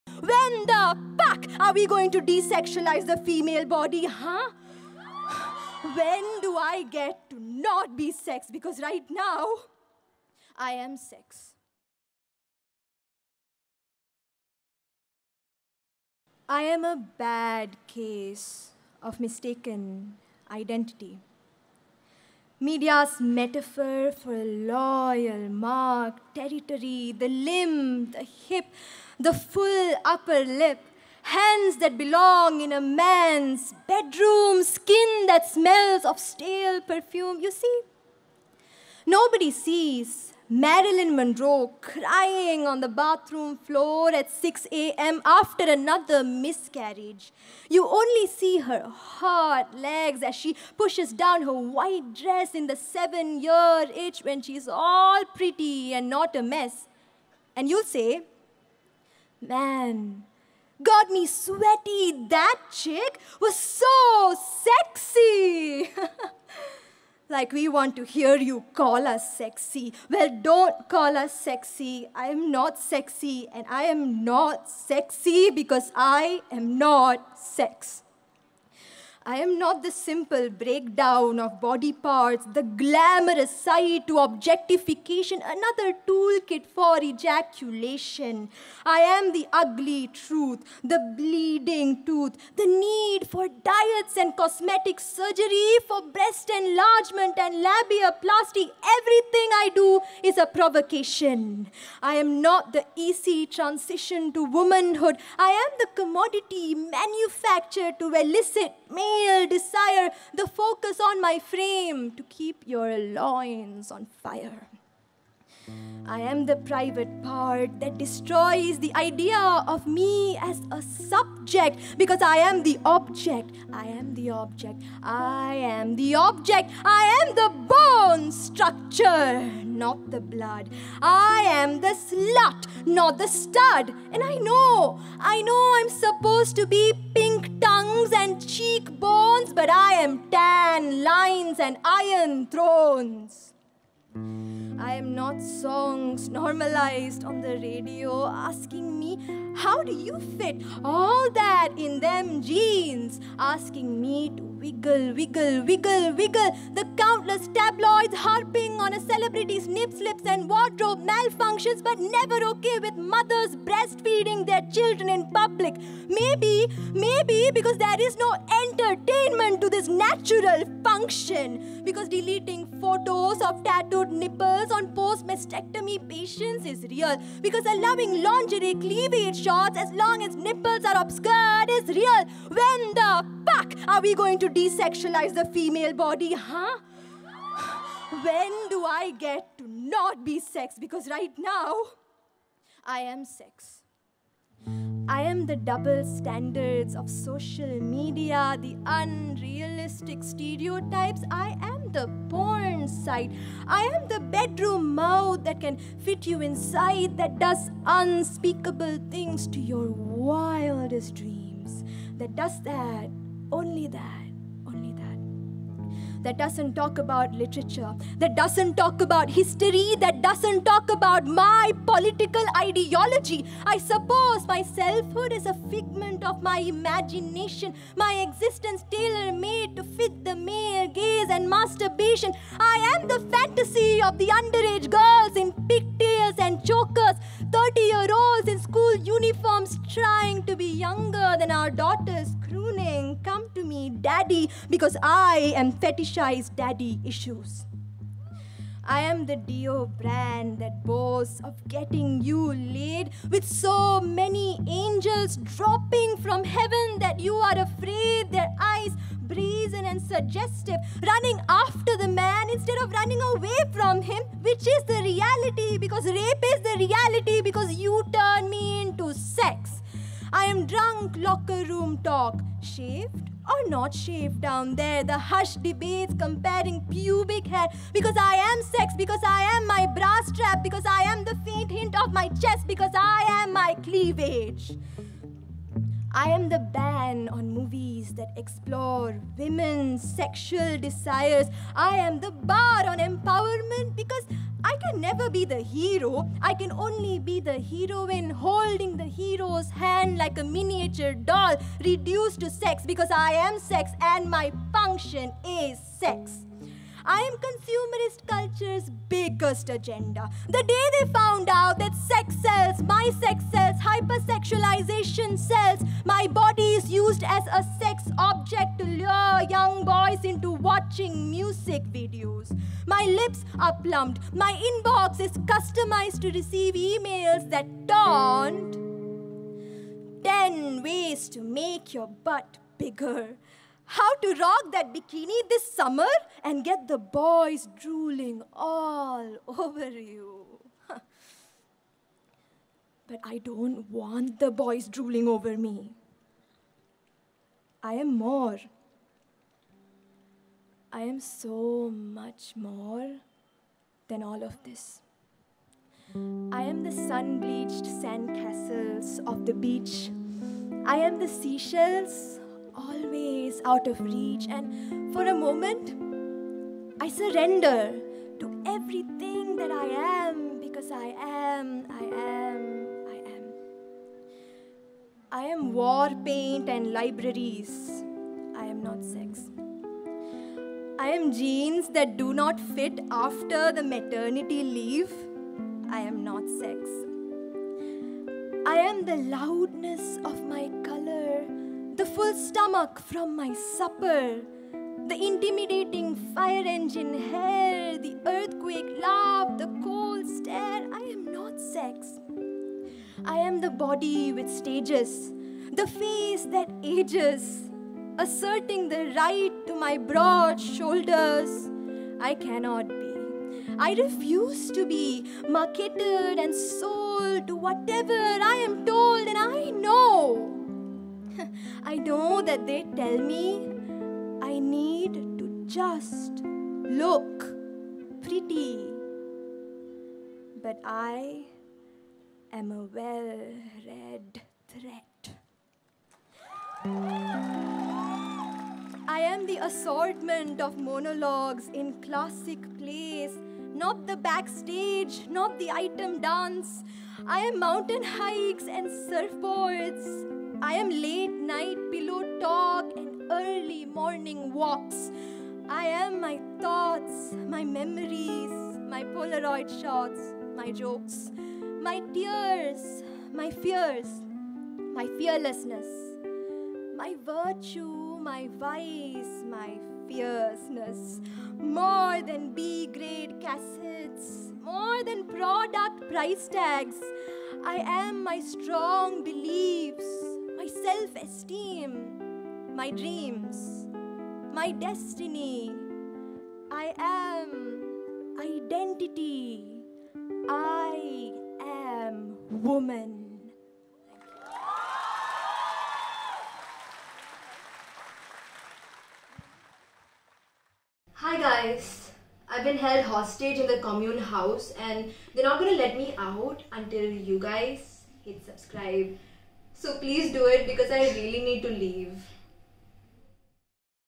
To All The Boys Who Call Me Sexy-Megha Rao Ft. Rounak Chawla|SpokenFest'20
As always Megha Rao is a force to reckon with. Her fiery response to women being objectified as sexual commodities forces the world to sit up, shut up and listen. "To All The Boys Who Call Me Sexy" is a message to the world that women are more than product price tags, item dances, bra straps or sex. Watch now!

Video Production: Adsto